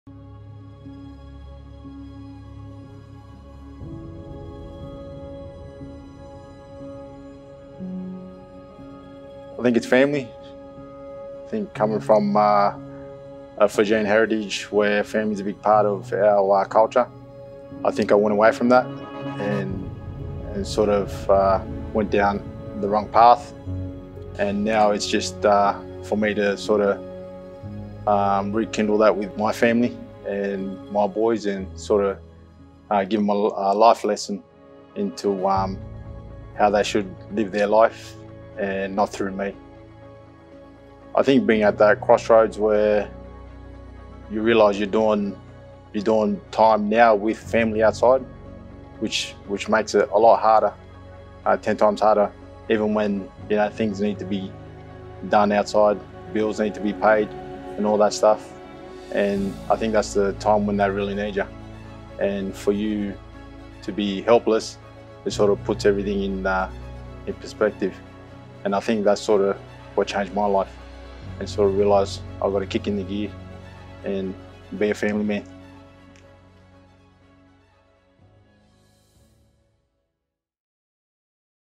LESSONS IN LIFE - JUNIOR NIUQILA
Growing up in the city in the 90’s and 00’s, and trying to escape the domestic violence at home there’s not many other options for a young kid but to be on the streets.

Junior possessed natural athletic abilities, but without the right mentors and guidance he spent over 11 years of his young adult life in and out of prison.

His athletic abilities were recognised even in prison and they allowed him to play Rugby league for the Redfern Warriors while he was still an inmate wearing a GPS ankle bracelet.

Juniors turnaround happened when he had his son and didn’t want him going down the same path that he did. Junior plays a mentoring role for troubled indigenous youth in Redfern and he specialises in boxing, endurance and conditioning.

Directed / edited by @zoku_creative and @liampatrick.jpg
Shot by @liampatrick.jpg
Follow @top_rated_ausfc
Follow @zoku_creative